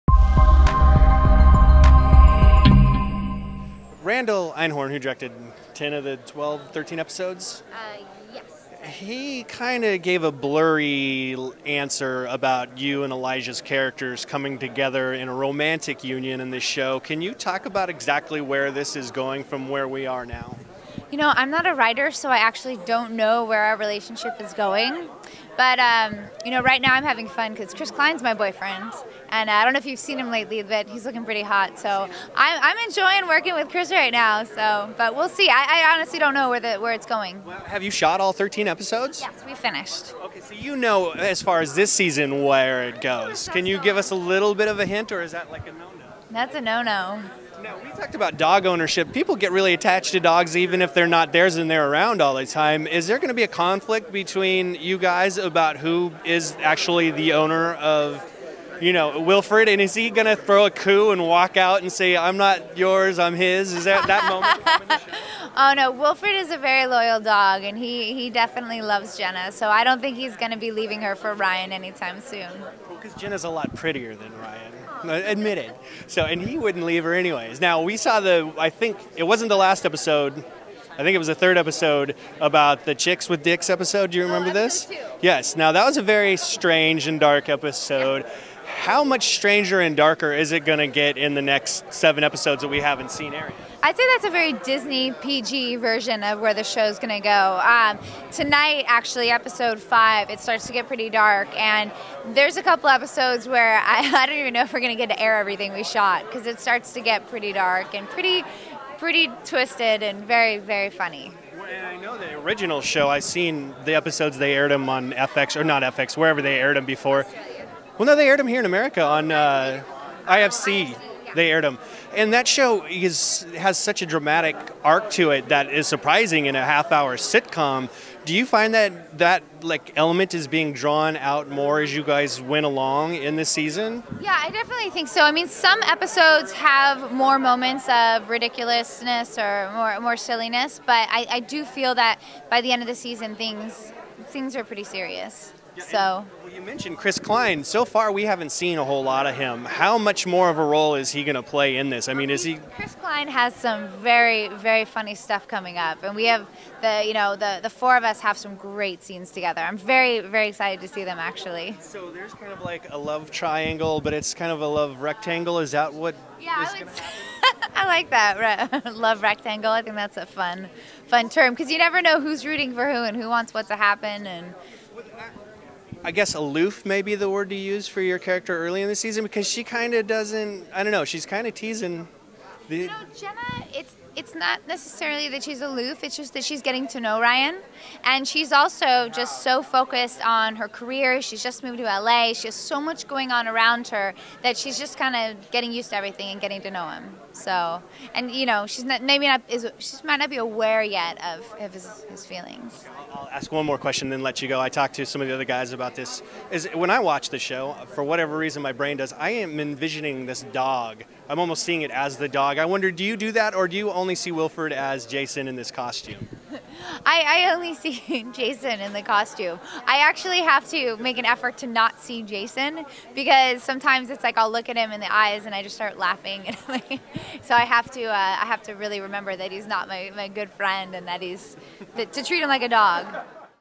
Wilfred - Season 1 Comic-Con Exclusive: Fiona Gubelmann
We go one-on-one with star Fiona Gubelmann to talk about the first season of Wilfred at Comic-Con 2011.